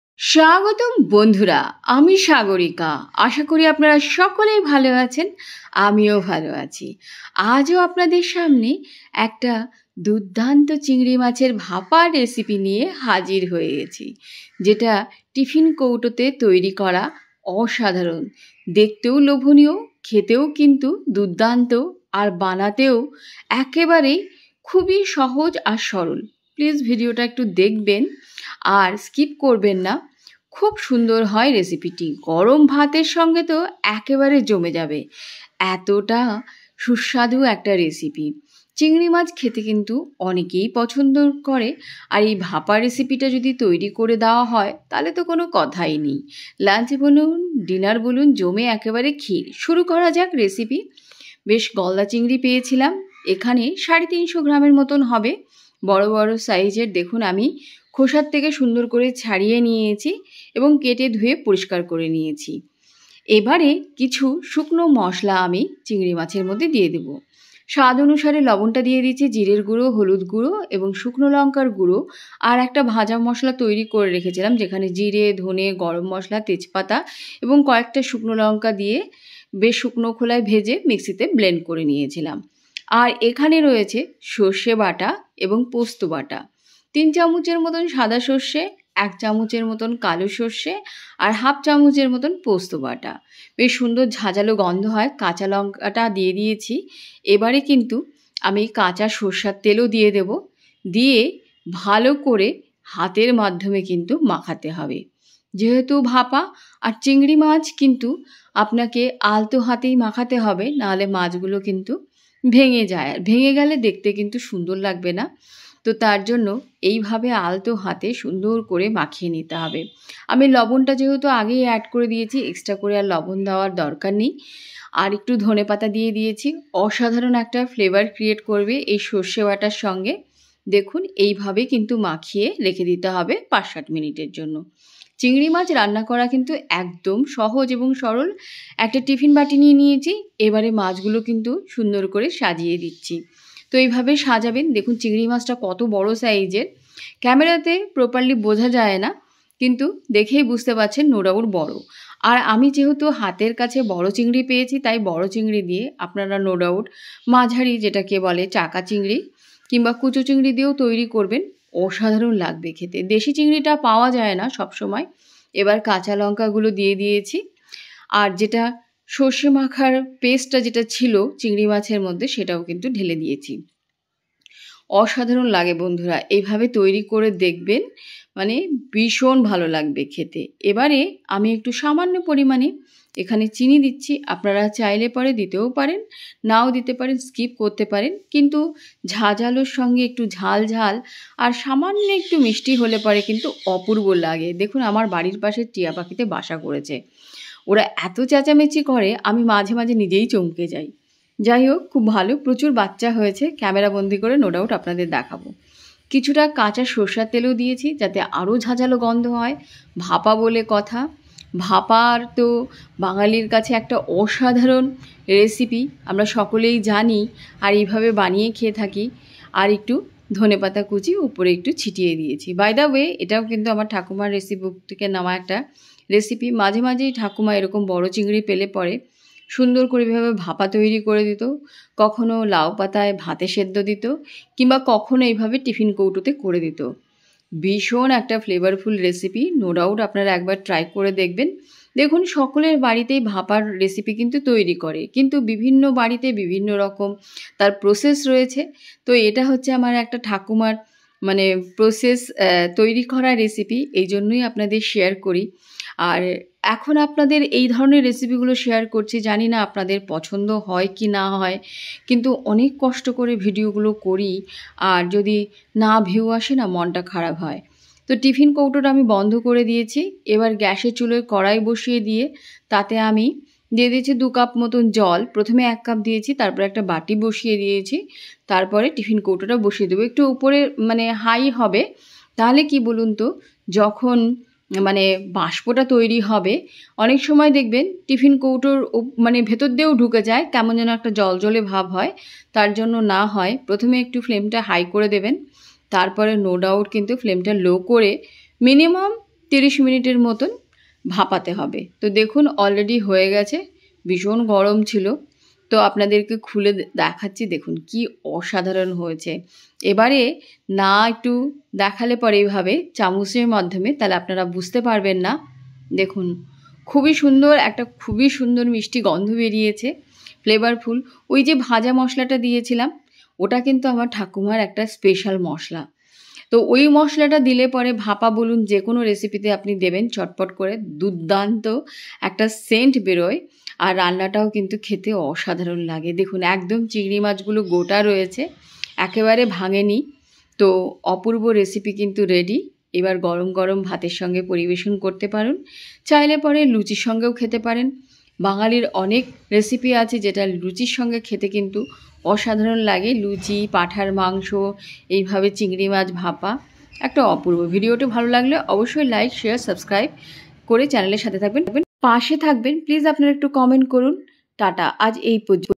টিফিন কৌটোয় চিংড়ি ভাপা😋নতুন স্বাদে একবার খেয়ে দেখুন😋👌chingri bhapa👍@sagarika cooking and vlogging
Tasty prawn recipe 😋👌 unique recipe